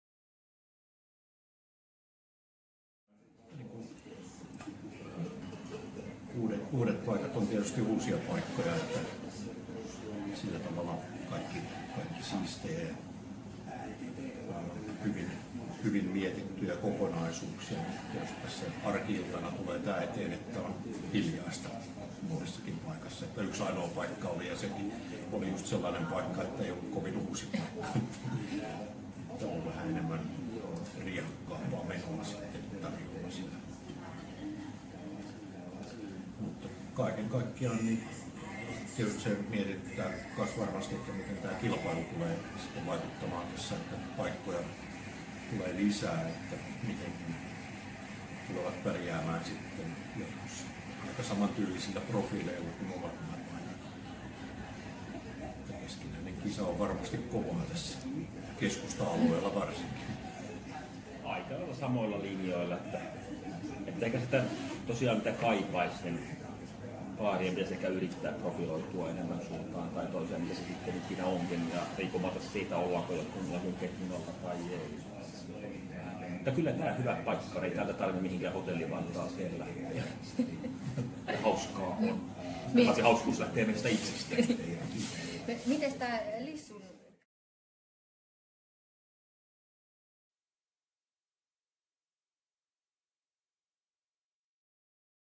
Yhteenveto
Baarikriitikot Kai Laine ja Juha Manninen kertovat, mitä Järvenpää-kierrokselta jäi käteen.